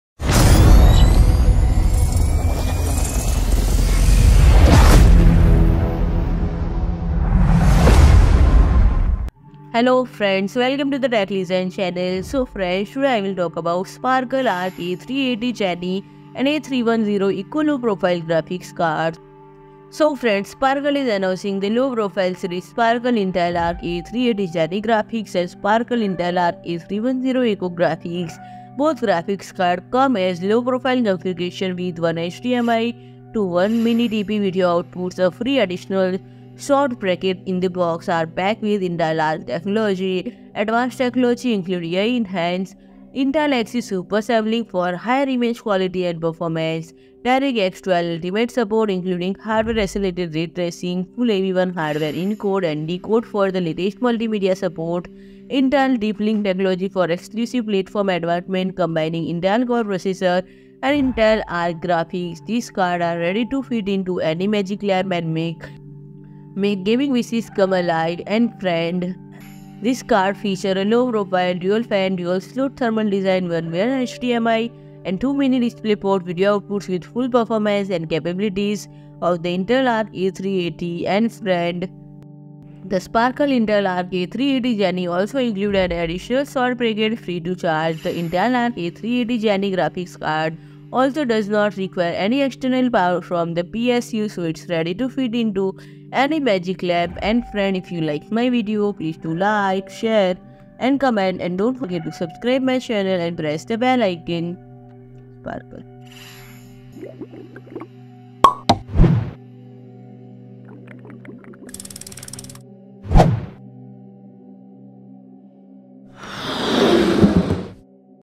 SPARKLE Arc A380 Genie And A310 Eco Low-profile Graphics Cards Launched - Explained All Details
PRODUCT BRAND STORE AMAZON

SPARKLE is announcing the low-profile series: SPARKLE Intel Arc A380 GENIE graphics and SPARKLE Intel Arc A310 ECO graphics. Both graphics cards come as low-profile configurations with 1x HDMI and 2x mini-DP video outputs, a free additional short bracket in the box, and are packed with Intel Arc technologies. Advanced technologies include AI-enhanced Intel Xe Super Sampling (XeSS) for higher image quality and performance, DirectX 12 Ultimate support including hardware-accelerated ray tracing, full AV1 hardware encode and decode for the latest multimedia support, and Intel Deep Link Technologies for exclusive platform advantages combining Intel Core processor and Intel Arc graphics. These cards are ready to fit into any magic lamp and make gaming wishes come alive! Furthermore, SPARKLE has built an exclusive Intel Arc A310 by successfully reducing the TBP (total board power) of the Intel Arc A310 from Intel's default 75 W to 50 W, providing the best balance of features, technologies and experiences in a small but advanced form-factor. This card features a low-profile, dual-fan, dual-slot thermal design with 1x HDMI and 2x Mini DisplayPort video outputs with full performance and capabilities of the Intel Arc A380. The Sparkle Intel Arc A380 GENIE also includes an additional short bracket free-of-charge. The Intel Arc A380 GENIE graphics card also does not require any external power from the PSU so it's ready to fit in any magic lamp. The SPARKLE team has successfully reduced the TBP of the Intel Arc A310 GPU from 75 W to 50 W, allowing the SPARKLE Intel Arc A310 ECO to be fully functional within a low-profile, single-slot, single-fan configuration. Meanwhile, the SPARKLE Intel Arc A310 ECO maintains all of the modern technologies and capabilities of the Intel Arc A310 GPU with 1x HDMI and 2x Mini DisplayPort video outputs as well as an additional short bracket free-of-charge. This card is designed to be the best choice for users seeking the best balance of modern technologies and capabilities for small form-factors.